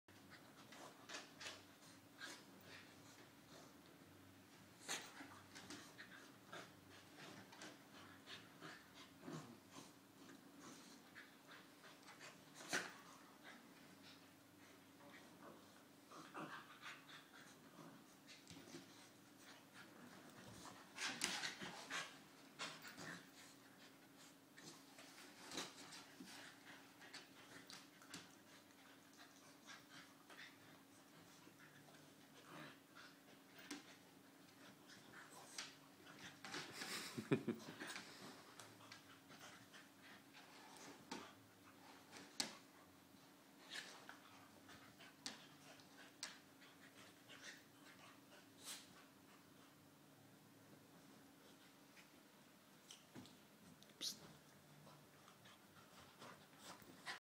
Don the Chow Chow vs Baxter the Jack Russell
Don & one of his best mates Baxter playing in the office. They can go on for hours these two.